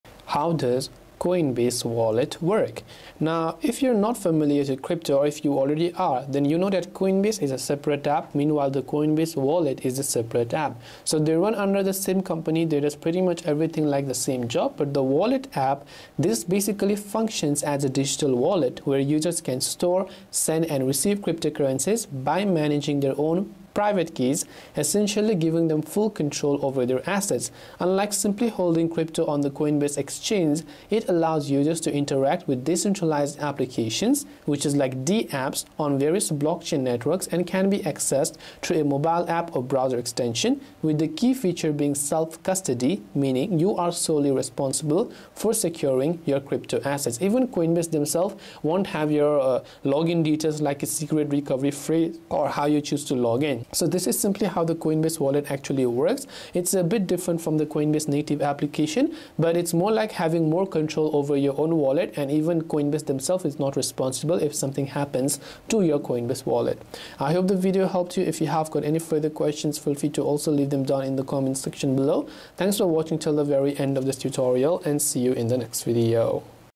How Does Coinbase Wallet Work !
The Coinbase Wallet is a powerful tool for managing your cryptocurrency, offering a decentralized approach to storing, sending, and receiving crypto assets. While it is developed by the same company that runs the Coinbase exchange platform, Coinbase Wallet operates as a separate app with distinct functionalities. Unlike the Coinbase exchange, which manages assets on behalf of users, the Coinbase Wallet provides self-custody of digital assets, meaning users retain full control of their private keys and funds.

This wallet is ideal for users who value control and security, as it allows you to independently manage your private keys without relying on third-party intermediaries. The app enables users to store a wide range of cryptocurrencies, from Bitcoin and Ethereum to various ERC-20 tokens, and interact with decentralized applications (DApps). Through its integration with multiple blockchain networks, users can easily engage in activities such as decentralized finance (DeFi), NFT transactions, and more.

A key feature of Coinbase Wallet is its self-custody design. This ensures that no one, not even Coinbase, has access to your login credentials or recovery phrases. It’s your sole responsibility to securely store your recovery phrase, as losing it could mean permanent loss of access to your assets. This heightened level of control appeals to users seeking privacy and autonomy but also requires a higher degree of responsibility.

The Coinbase Wallet can be accessed via a mobile app or a browser extension, providing flexibility in managing your crypto on the go or from your desktop. Its user-friendly interface makes it accessible even for those who are new to the crypto space, while its robust features cater to more advanced users.

Timestamps:
0:00 Introduction to Coinbase Wallet
0:03 Difference between Coinbase and Coinbase Wallet
0:11 Overview of Coinbase Wallet functionality
0:18 Managing private keys and self-custody
0:30 Interacting with decentralized applications (DApps)
0:41 Accessing Coinbase Wallet through apps and extensions
0:54 Importance of securing your recovery phrase
1:07 Conclusion